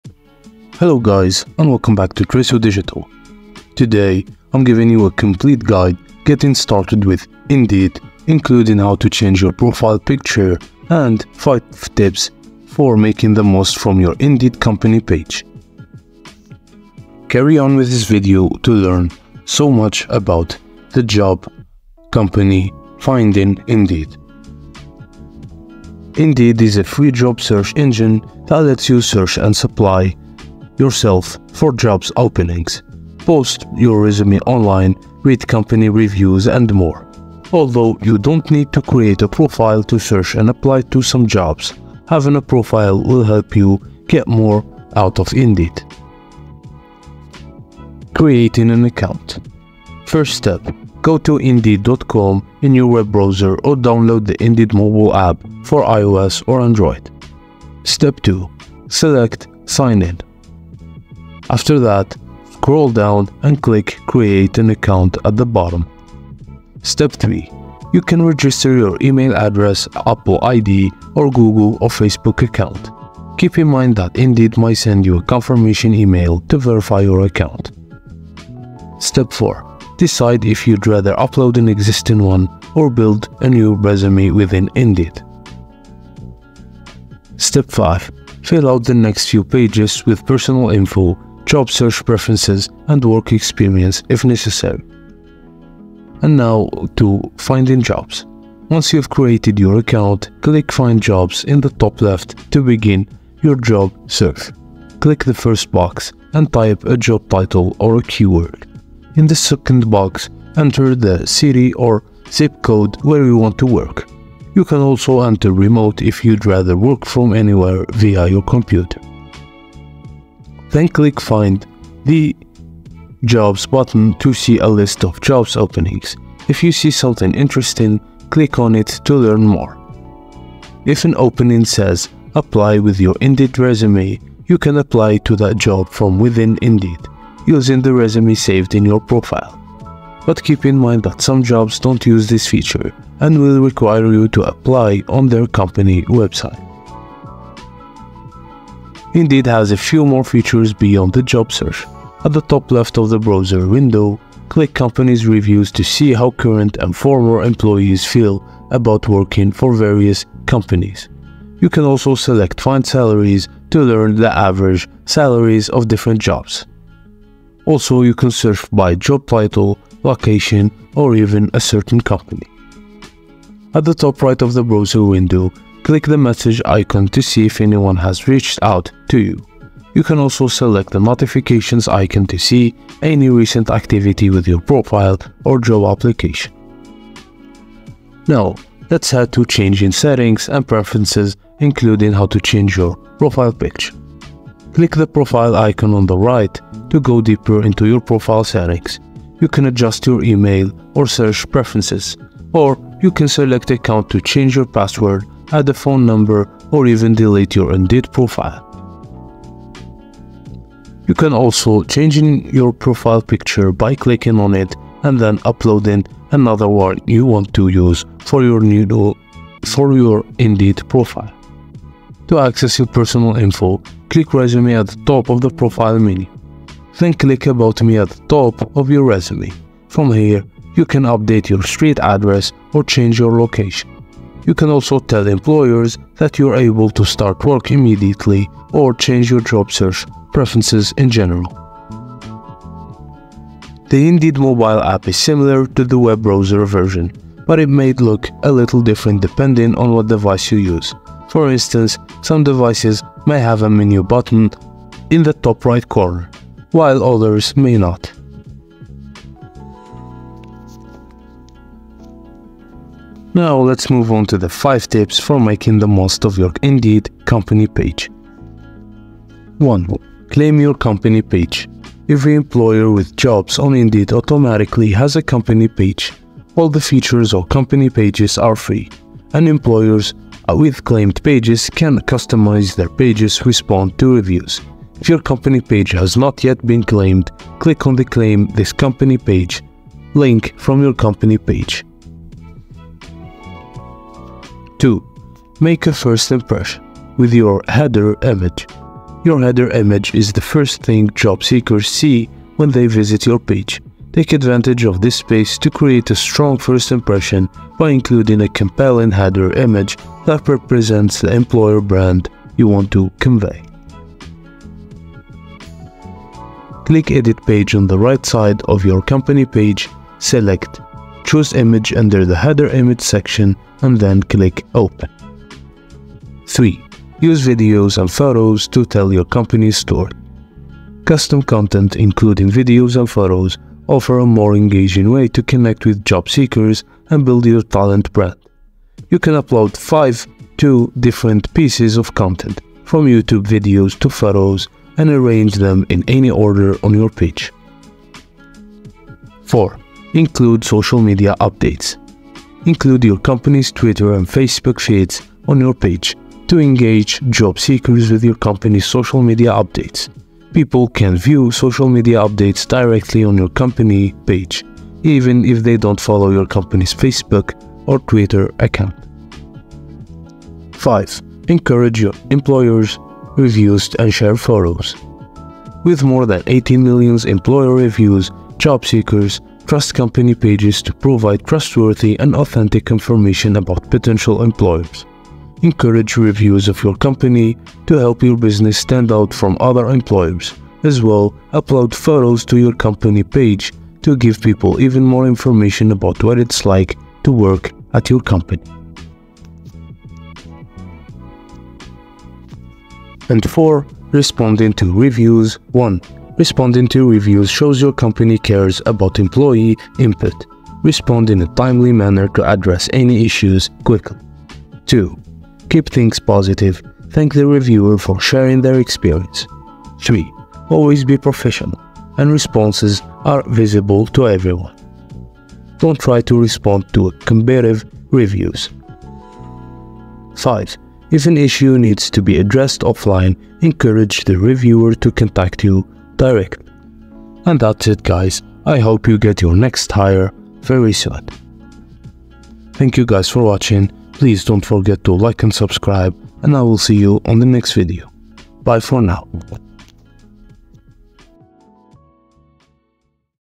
How To Change Profile Picture in Indeed - Full Guide (2025)
In this video I will show you how you can change the profile picture in indeed.